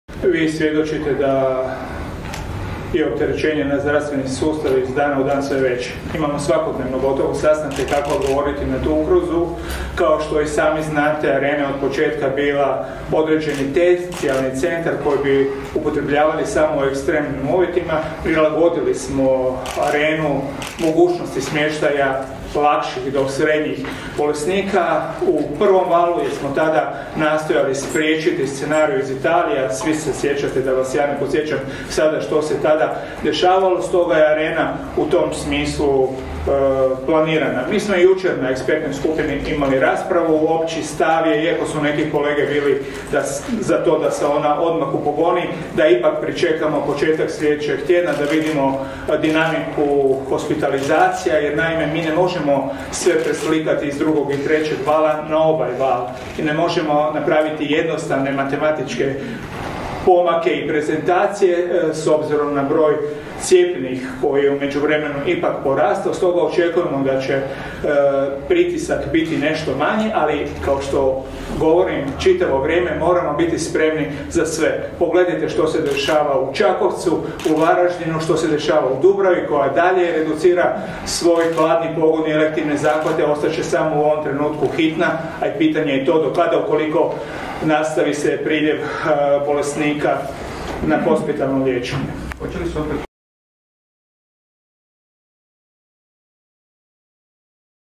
Vili Beroš o ponovnom aktiviranju Arene Zagreb
Prije sjednice Užeg kabineta Vlade izjavu za medije dao je Vili Beroš koji je govorio o mogućnosti ponovne aktivacije Arene Zagreb zbog pogoršanja epidemiološke situacije.